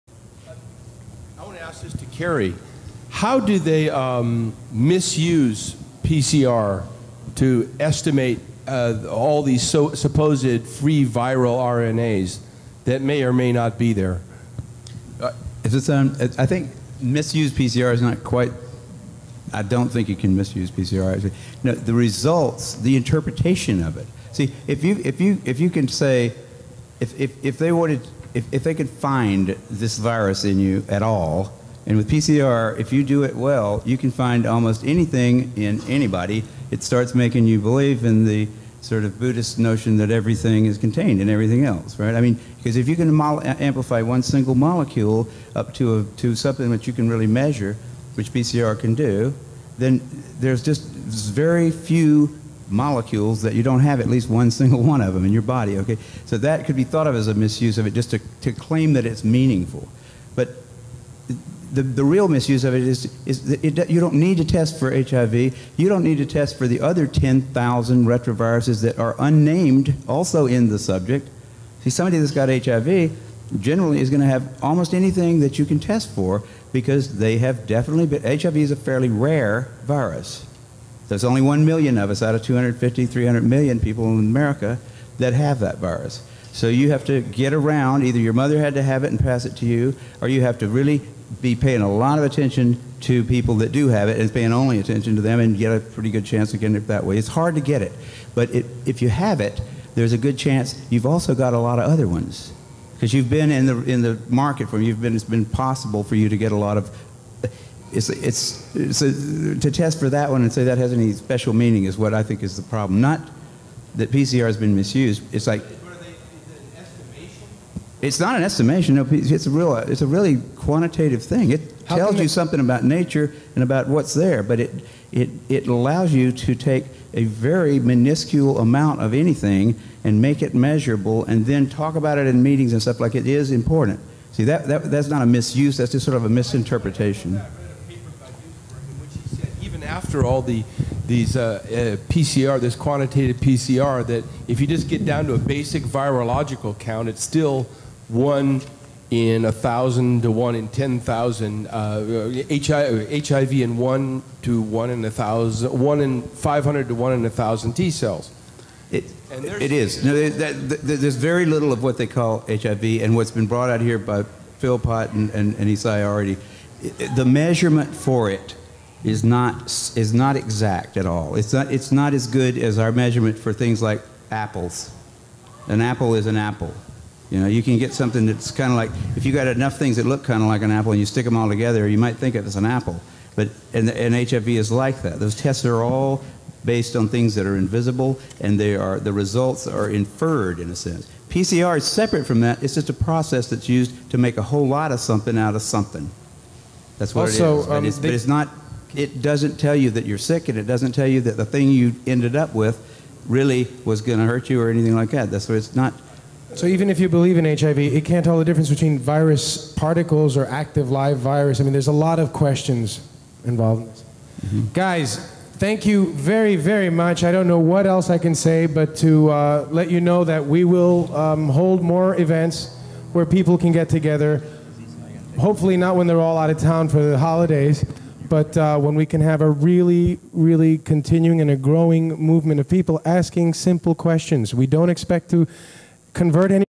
Kary Mullis (Inventor of PCR) Explains the PCR Test
Inventor of PCR is Kary Mullis has also done a lot of research on HIV. In this youtube he states that 'presence of a lump of RNA' does not mean 'sick or contagious'. RNA chunks can be present in our body for up to weeks. He also says that PCR is so powerful that single molecules sufficient to be amplified by PCR. Using PCR without the presence of clinical complaints is absolutely not recommended.

Thank you everyone of you.